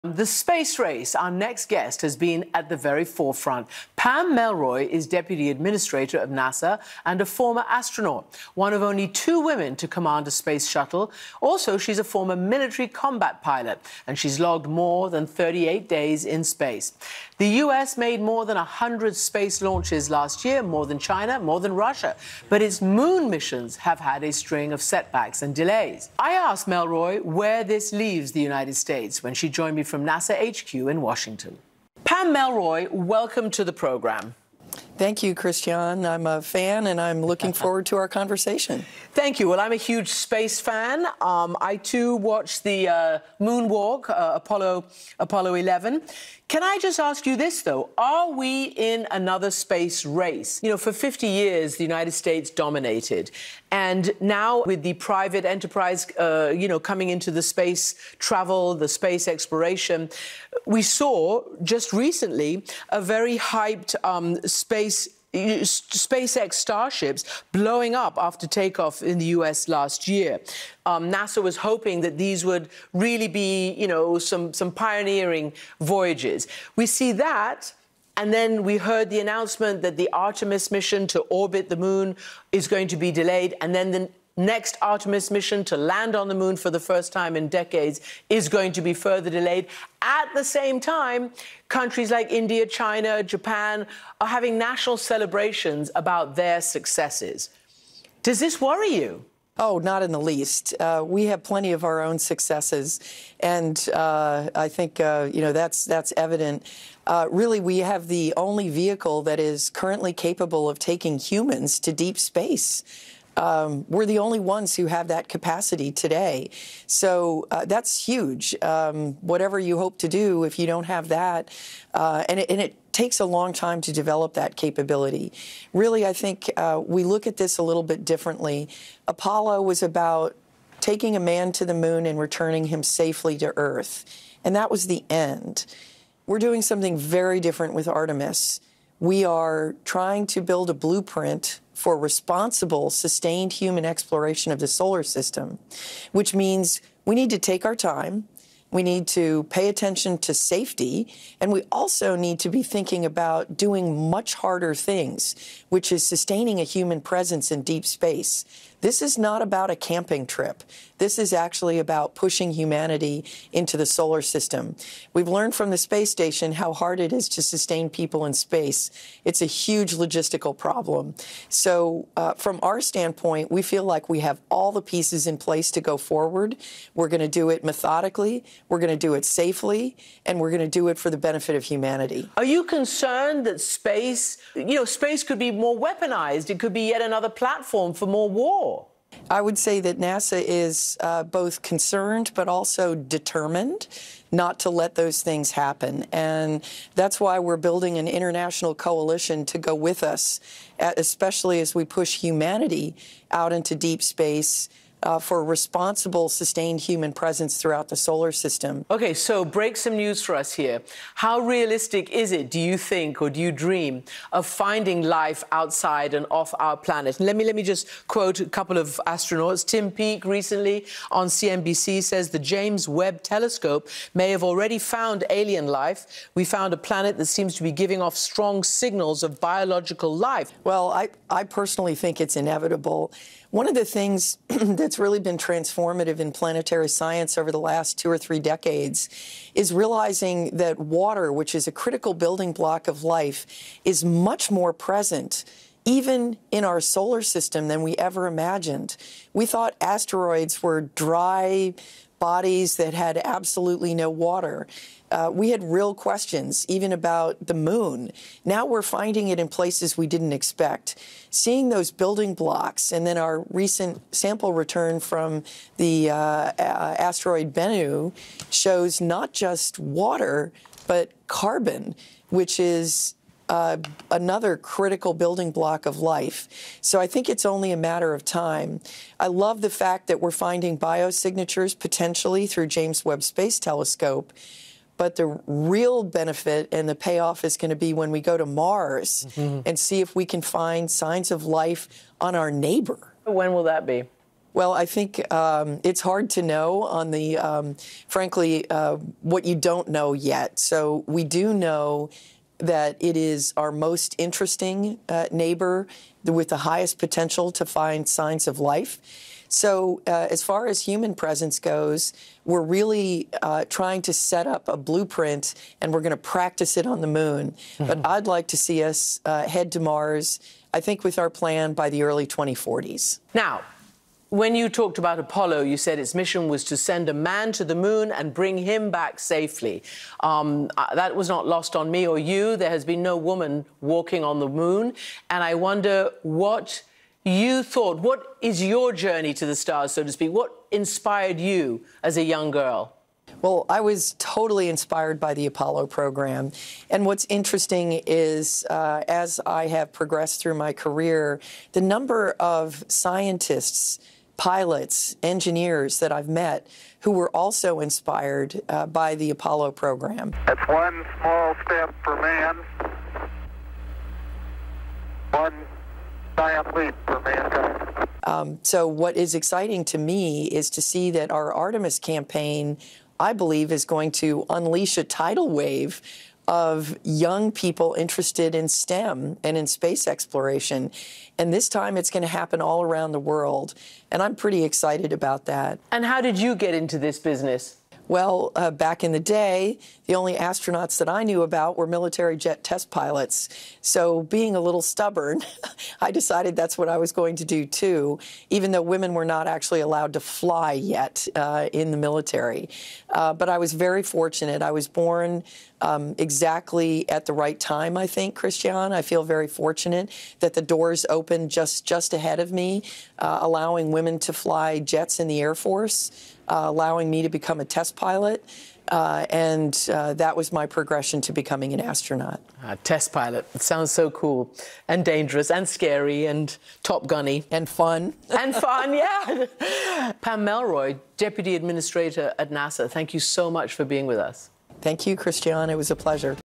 Could space be the next platform for war? Hear what NASA official thinks
CNN's Christiane Amanpour speaks with NASA deputy administrator Pamela Melroy about the potential that space could be the next platform for war between nations.